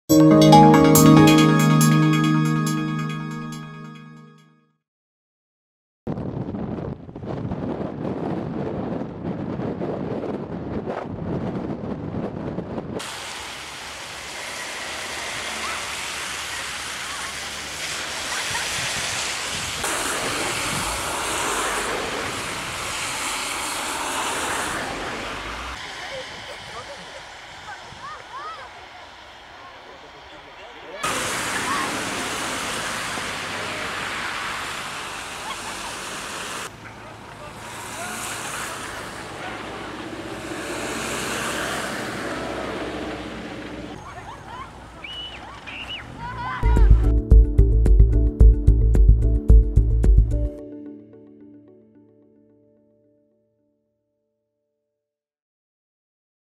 قسنطينة ترتدي حلة بيضاء بتساقط الثلوج والمواطنون يخرجون للاستمتاع
قسنطينة ( وأج) - سجلت المناطق التي يزيد علوها عن 700 متر بمنطقة قسنطينة تساقط كميات معتبرة من الثلوج ، الأمر الذي دفع بالكثير من المواطنين إلى الخروج للاستمتاع بالثلوج رغم صعوبة السير في العديد من المحاور.